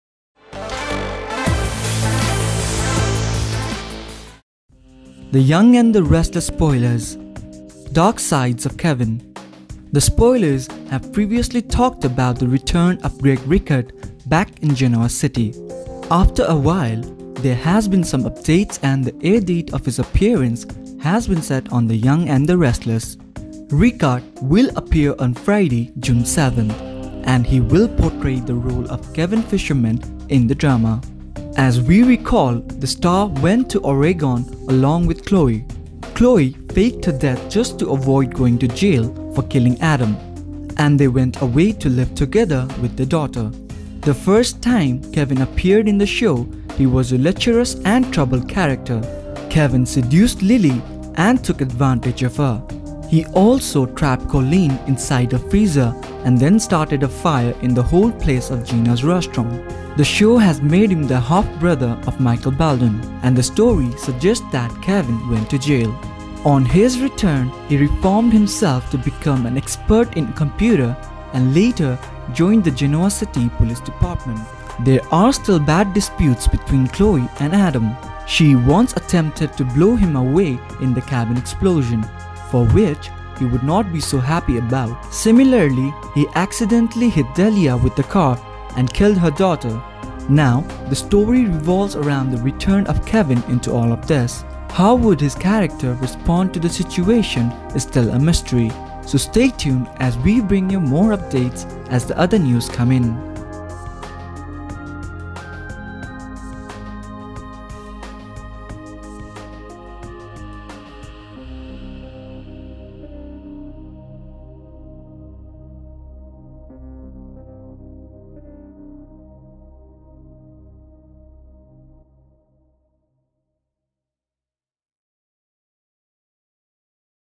Young and The Restless Spoilers: Dark Sides of Kevin
The Young and the Restless spoilers: Return of Kevin Fisher back in the show. Greg Rikaart will appear on June 7, 2019, to portray the role of Kevin Fisher. Will the fans see the dark sides of Kevin yet again?